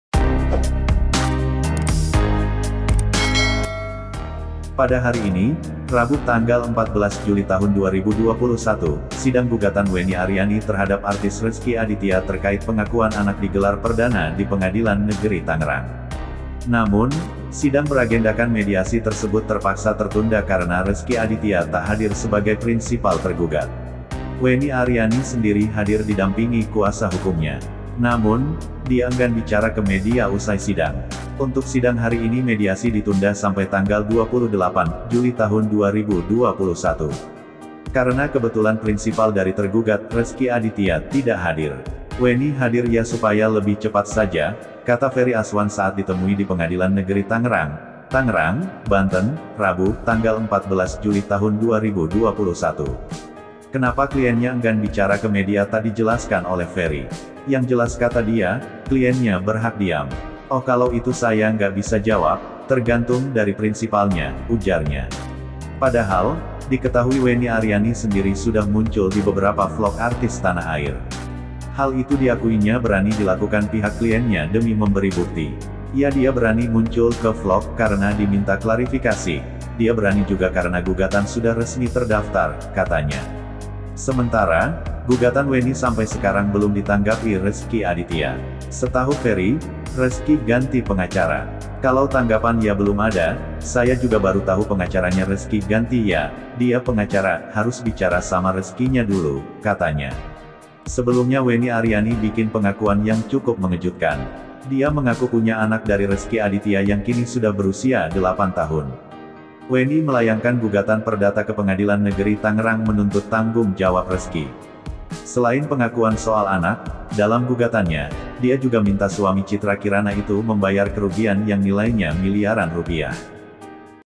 PANTANG MUNDUR!!Rezky Aditya Tak Hadir di Sidang Pengakuan Anak Wenny Ariani
Pada hari ini, Rabu (14/7/2021), sidang gugatan Wenny Ariani terhadap artis Rezky Aditya terkait pengakuan anak digelar perdana di Pengadilan Negeri Tangerang.

Namun, sidang beragendakan mediasi tersebut terpaksa tertunda karena Rezky Aditya tak hadir sebagai prinsipal tergugat. Wenny Ariani sendiri hadir didampingi kuasa hukumnya. Namun, dia enggan bicara ke media usai sidang.